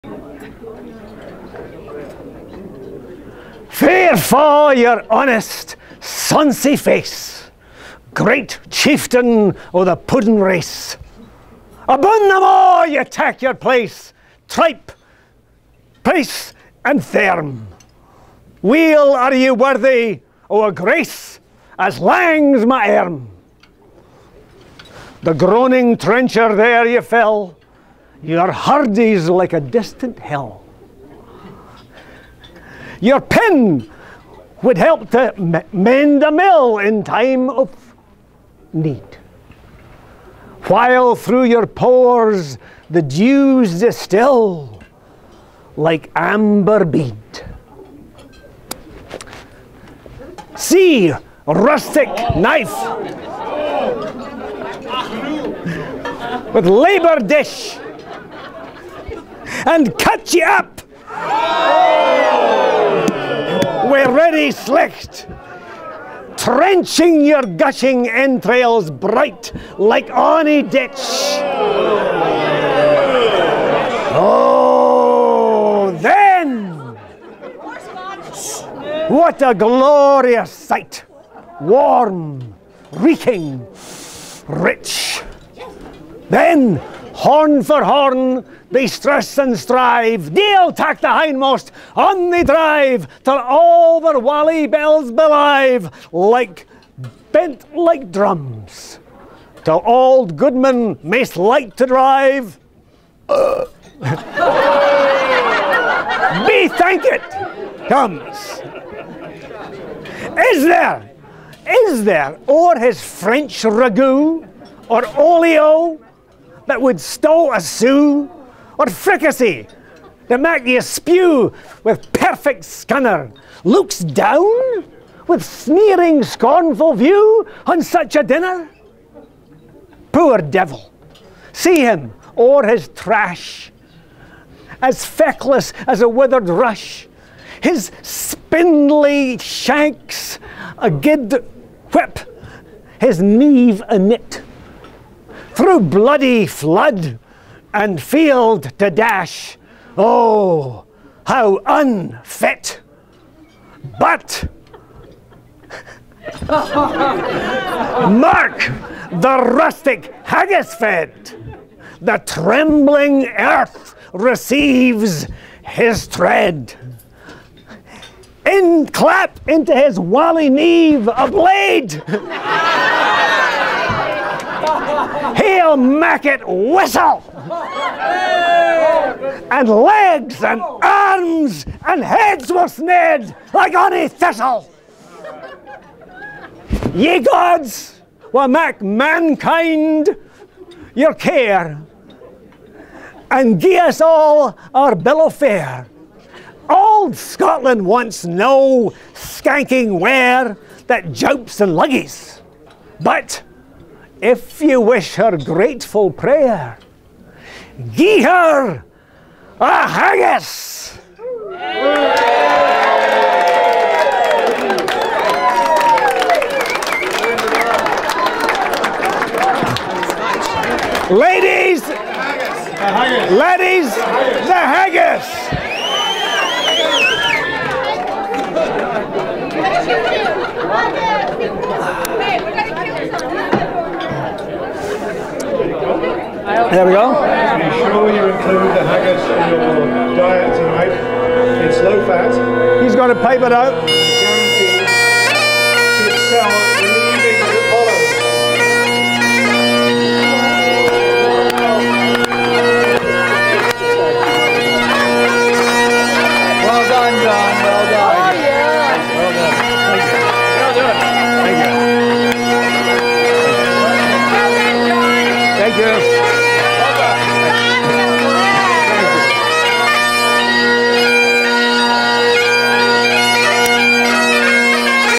Address To A Haggis
John Plank delivers The Address To A Haggis at the Toronto Lawn Tennis Club in Toronto.  John Plank is one of the most highly qualified and respected communications coaches in Canada. He coaches National Leaders and Lawyers in the public and corporate sectors throughout Canada, the US and the UK.